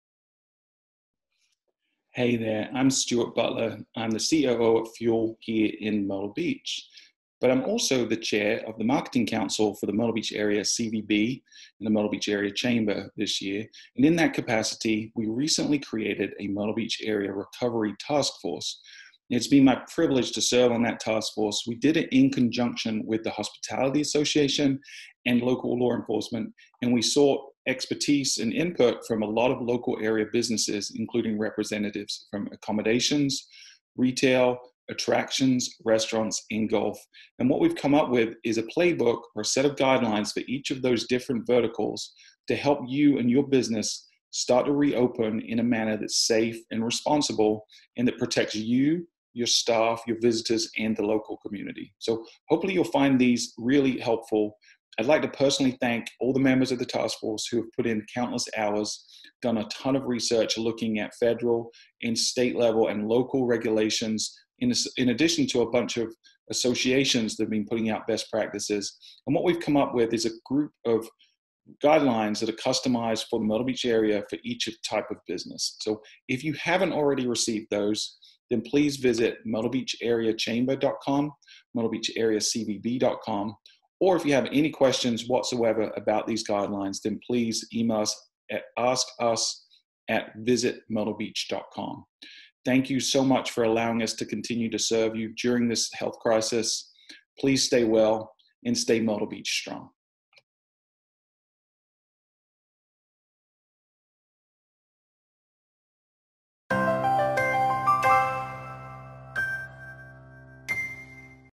Accelerate Myrtle Beach Task Force Shares Guides to Help Grand Strand Businesses Prepare to Reopen
The Accelerate Myrtle Beach Recovery Task Force was created by the Myrtle Beach Area Chamber of Commerce and Convention and Visitors Bureau in collaboration with the Myrtle Beach Area Hospitality Association to help facilitate the economic recovery of the Grand Strand area by providing guidelines for marketing and communications to local area accommodations, restaurants, attractions, golf courses, retail outlets, and other businesses.

TASK FORCE REPRESENTATION
Marketing Council Chairman – Stuart Butler
Chamber President and CEO – Karen Riordan
Hospitality Association President and CEO – Stephen Greene
Lodging – Matthew Brittain, Bob Barenberg, Matt Klugman
Restaurants – Heidi Vukov
Retail – Jim Powalie
Golf Courses – Dave Genevro
Attractions/Entertainment Venues – Robert Stinnett, Pablo Chavez, Mark Lazarus, David Olive
Myrtle Beach Police Department – Chief Amy Prock and Captain Eric DiLorenzo